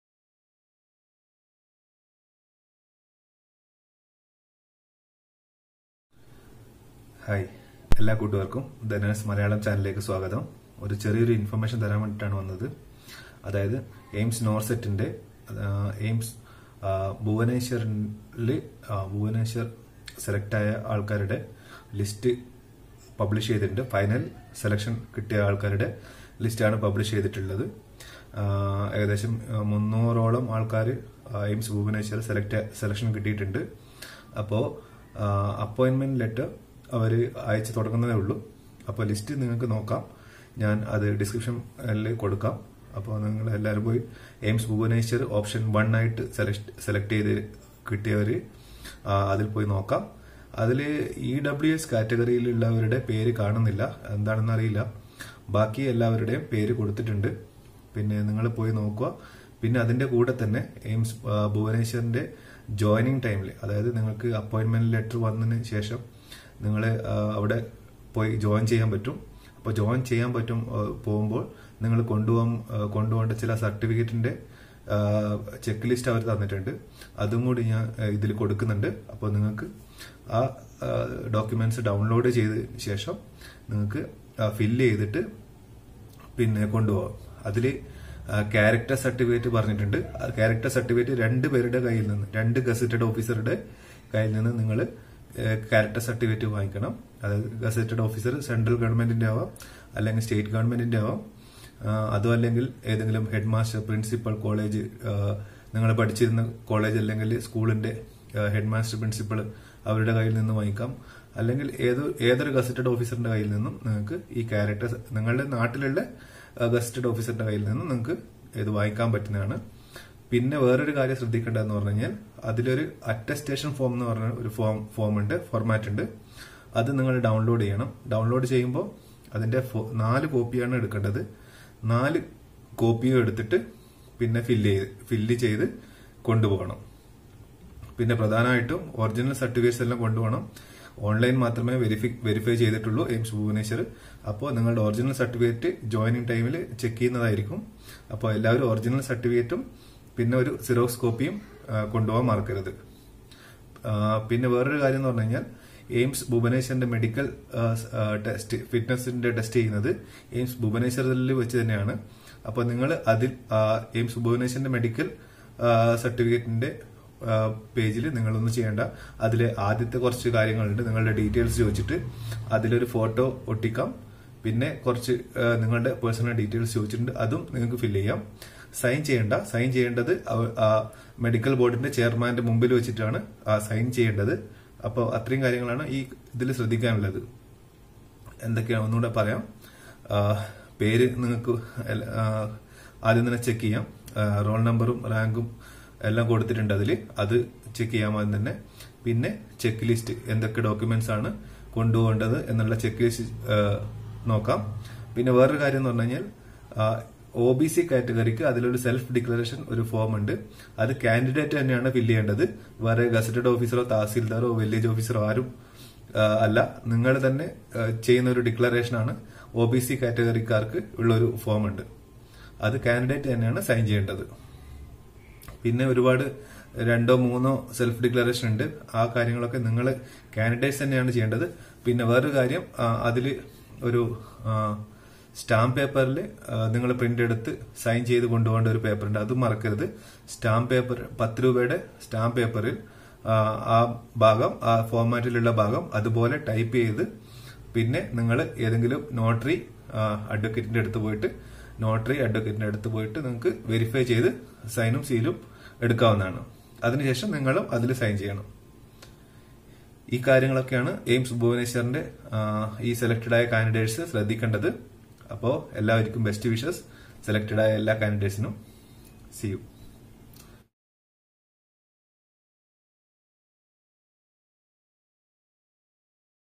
AIIMS NORCET News - AIIMS Bhubaneswar Final Selection List Published
AIIMS NORCET new updates came, that AIIMS Bhubaneswar published nursing officer final selection list after online verification of documents. The appointment letters may receive soon.Click here for the selected candidate's list and document Check List